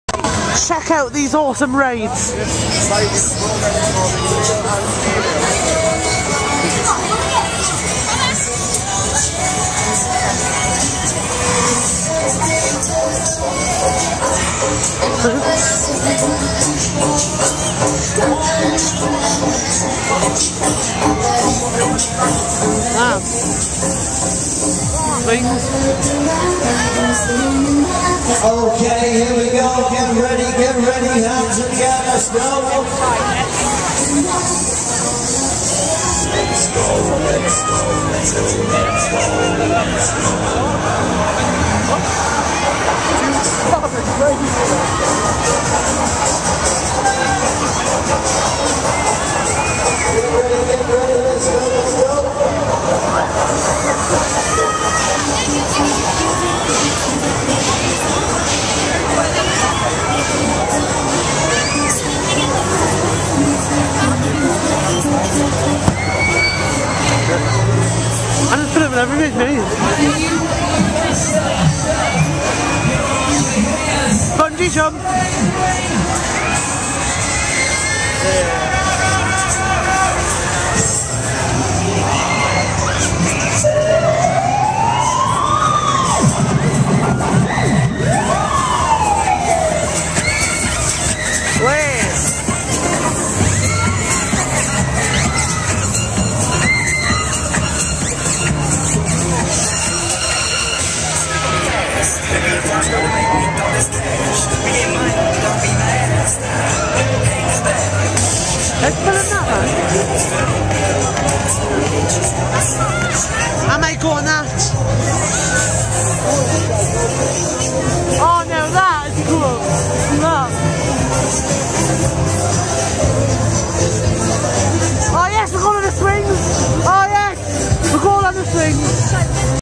Exploring The Hoppings Fayre 2011
The Hoppings, Uk;s biggest and craziest fayre, 2011 exploration :)
Dig all these awesomesauce rides!!